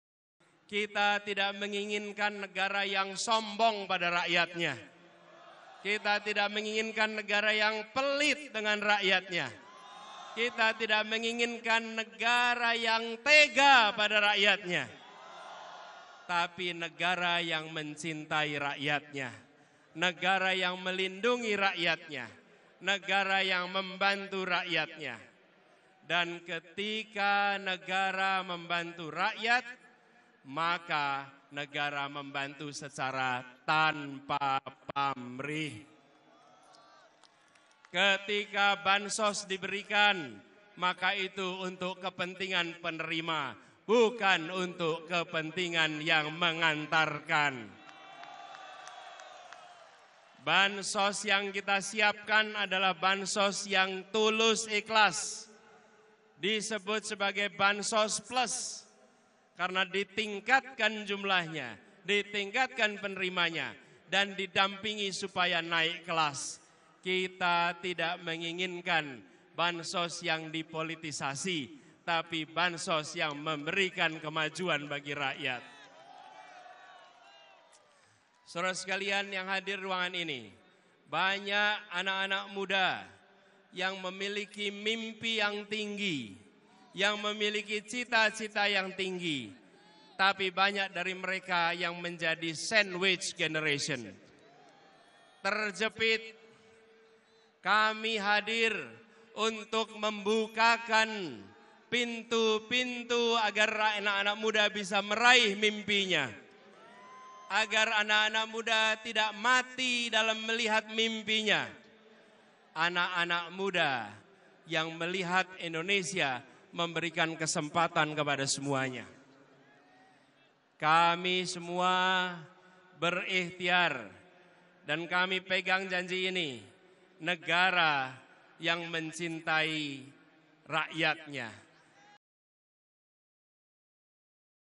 ANIES SIAP BUAT BANSOS ‘NAIK KELAS’ DAN BANTU SANDWICH GENERATION
Calon Presiden Nomor Urut 01 Anies Baswedan menyinggung mengenai bantuan sosial (bansos) yang dinilai telah dipolitisasi pihak tertentu. Ia memahami bansos memang penting, namun seharusnya difokuskan pada penerimanya, bukan kepentingan yang memberi.

Jika terpilih menjadi presiden, Anies berjanji akan membuat bansos 'naik kelas' dan difokuskan pada penerimanya. Selain soal bansos, Anies juga menyinggung mengenai banyaknya anak-anak muda yang memiliki cita-cita tinggi di Indonesia, namun terhambat karena menjadi sandwich generation.